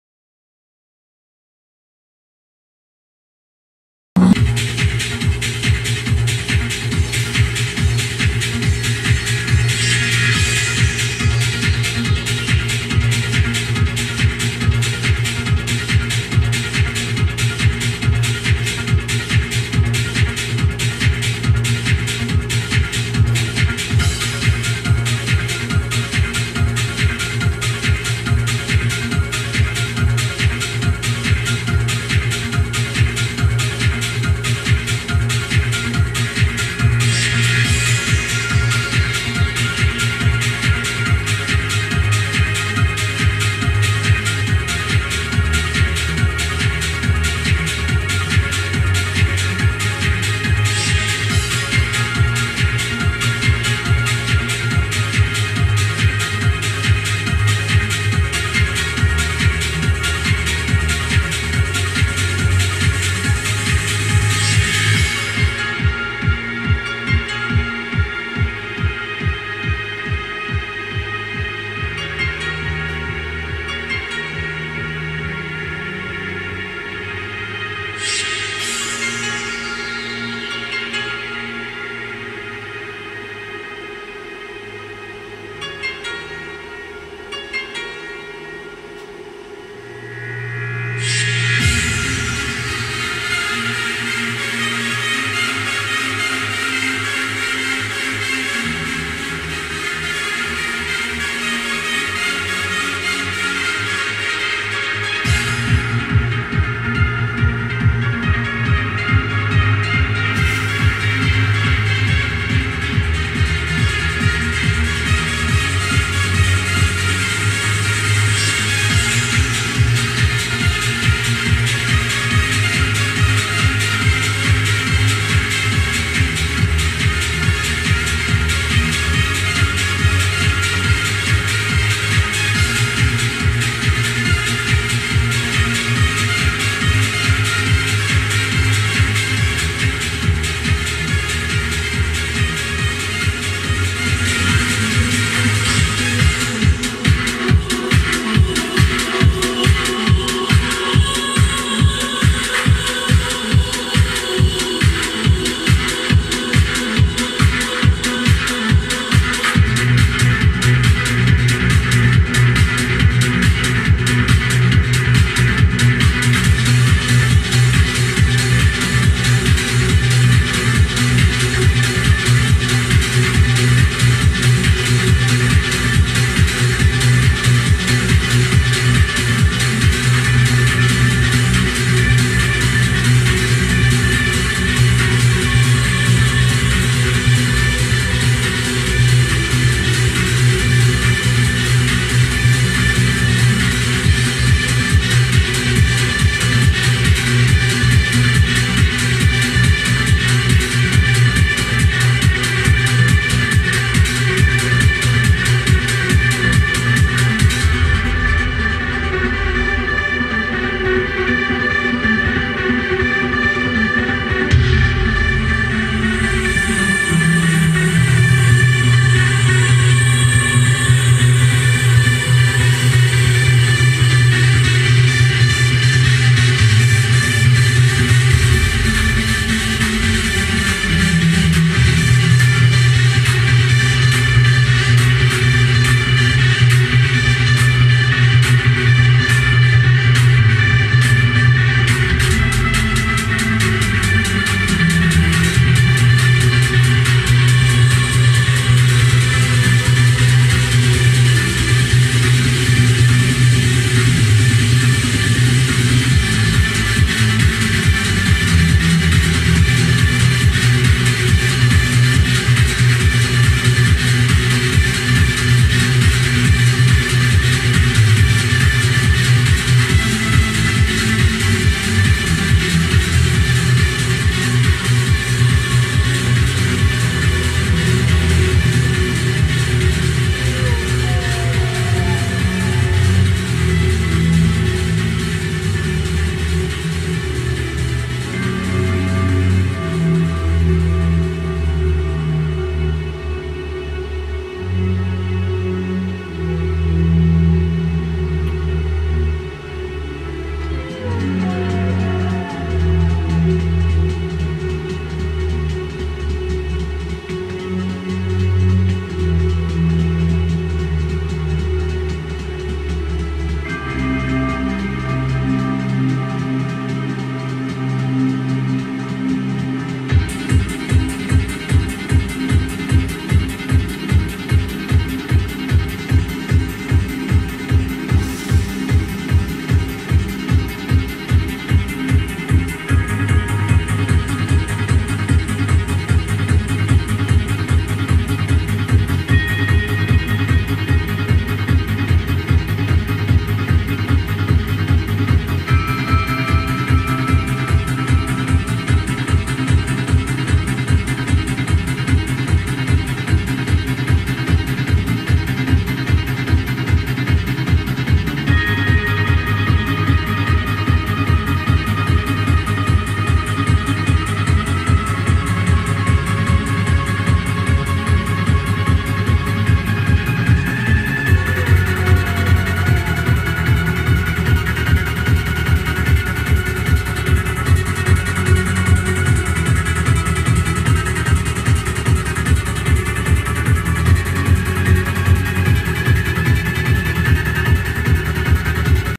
MX-4 lightshow.wmv
Playing around with Martin Lightjockey live with a set of 4 Martin MX-4s to some dance music.

If your liking what you see, you can book me to do the lighting for your event in the north west area of england.